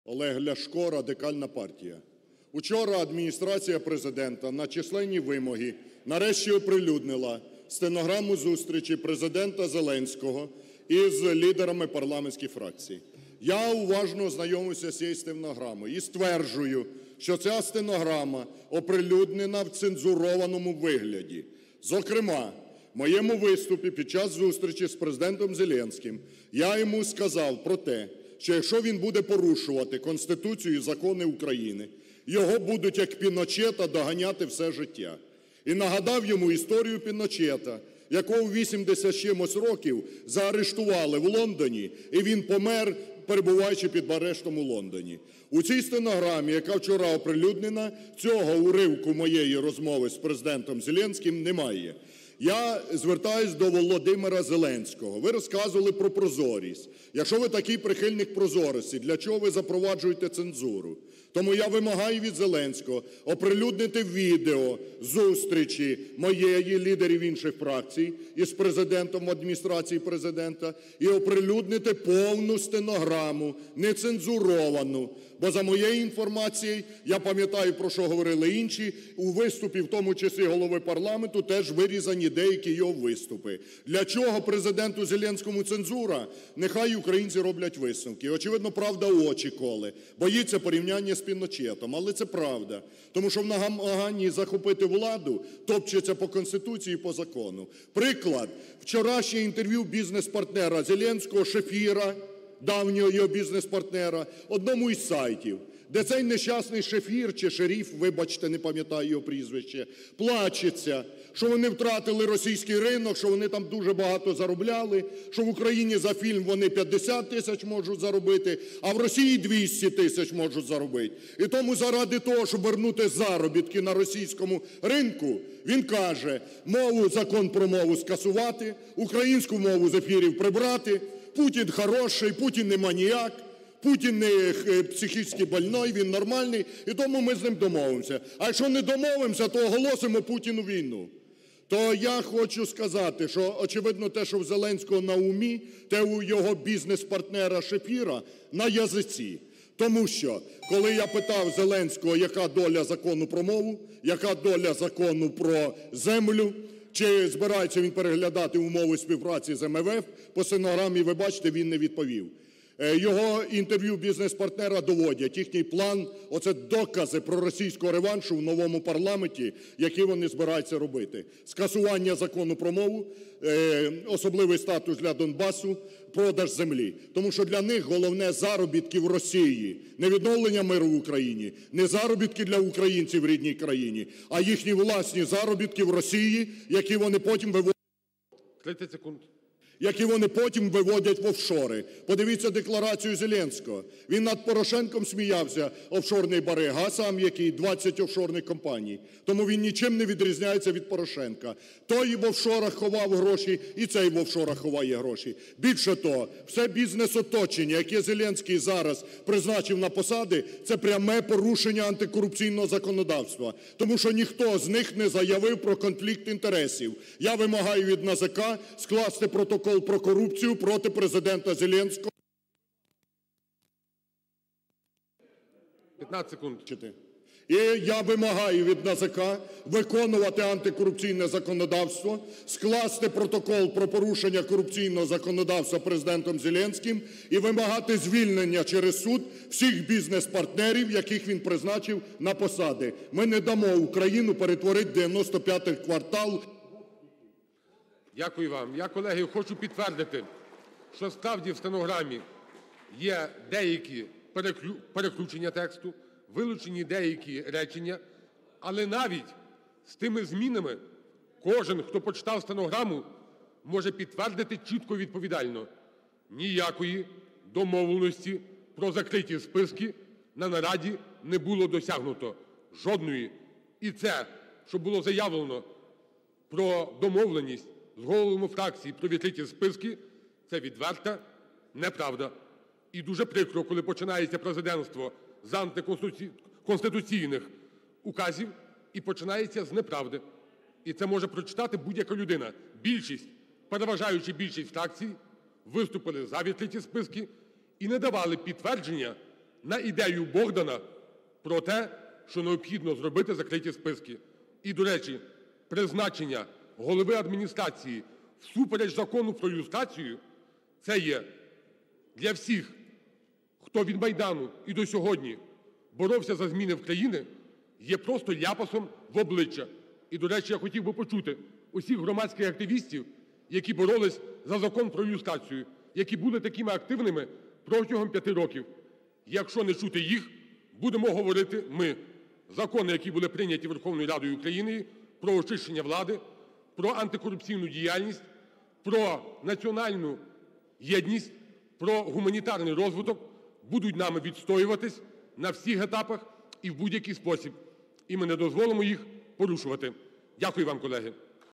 Ляшко і Парубій заявили про цензуру в стенограмі з консультацій в АП
Команду Зеленського турбує не питання миру в Україні, а питання відновлення своїх заробітків і бізнесу в Росії. Про це заявив з трибуни парламенту Олег Ляшко. Заради цього, стверджує політик, новий Президент і його оточення готові скасувати закон про мову і ухвалити спеціальний статус для Донбасу. Однак Ляшко запевнив: поки він у парламенті - проросійського реваншу не буде.
«В інтерв’ю бізнес-партнер Зеленського Шефір плакався, що вони втратили російський ринок, на якому дуже багато заробляли. Казав, що в Україні за фільм вони можуть отримати 50 тисяч, а в Росії – 200 тисяч. І тому заради повернення заробітків на російському ринку він каже, що «треба скасувати закон про мову, прибрати українську мову з ефірів, Путін не маніяк, тому ми з ним домовимося. А якщо не домовимося – оголосимо Путіну війну». Що у Зеленського на умі – те в його бізнес-партнера Шефіра на язиці! І підтвердженням цього є те, що, коли я питав Зеленського про долю законів про мову, землю, співпрацю з МВФ, він не відповів», - підкреслив Ляшко.
Лідер РПЛ також наголосив, що Зеленський за кількістю офшорних кампаній не поступається Порошенку і його оточенню.
«Подивіться на декларацію Зеленського. Він сміявся над Порошенком, казав, що той офшорний барига, а в самого 20 офшорних компаній. Порошенко в офшори ховав гроші – і Зеленський в офшорах ховає гроші. І це те, чим вони планують займатися в новому парламенті. Скасування закону про мову, особливий статус для Донбасу, продаж землі – все для того, щоб не втратити російський ринок. Для них головне - заробітки в Росії, а не відновлення миру в Україні, робота і добробут українців в рідній країні», - вважає Ляшко.